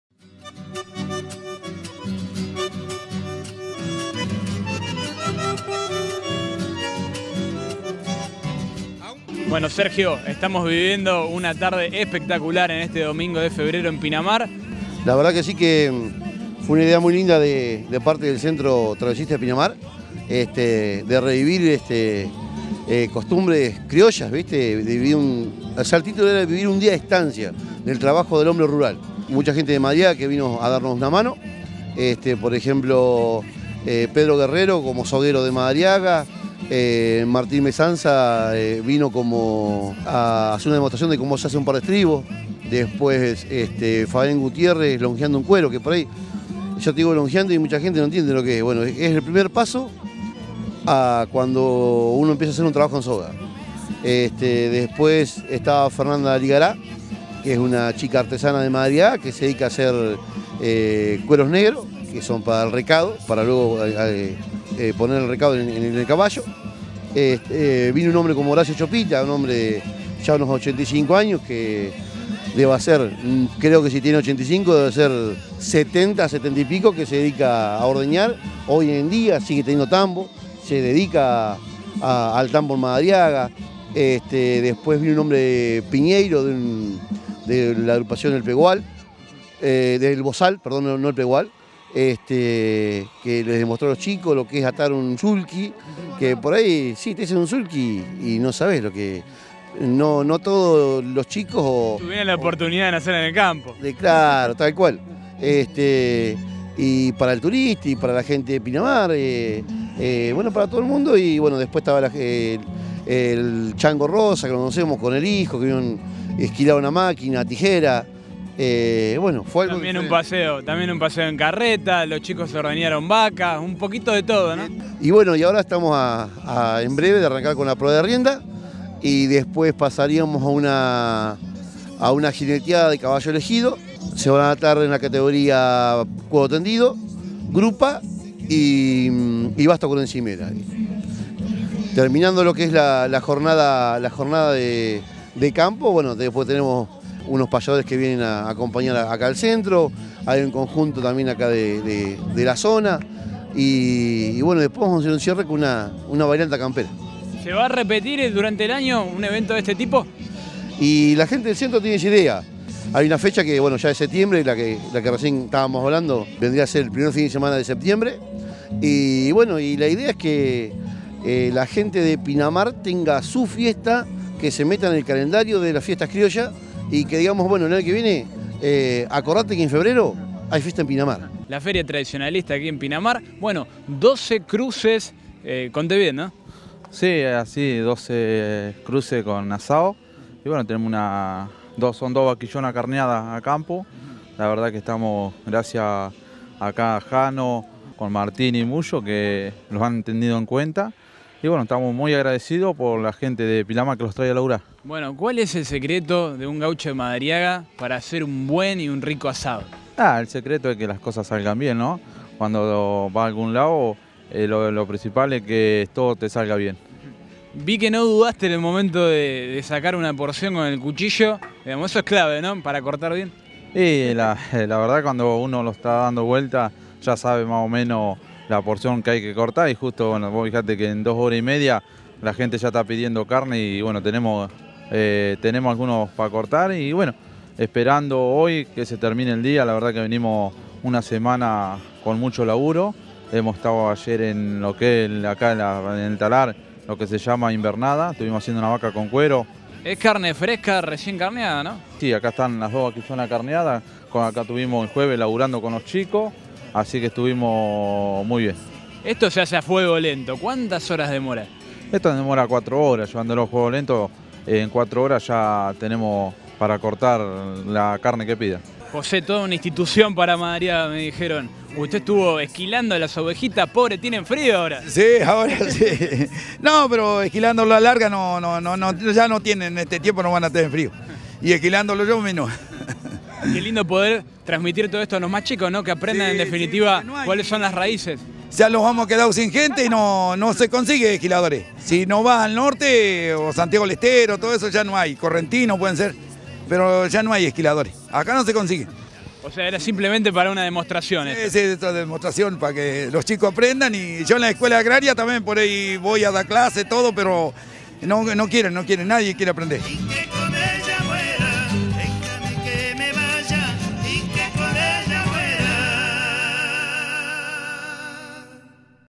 Fiesta en el Centro Tradicionalista
La fiesta se desarrollo el pasado fin de semana, con la finalidad de enseñar parte de nuestra tradición campestre. Un día con animales y comida típica.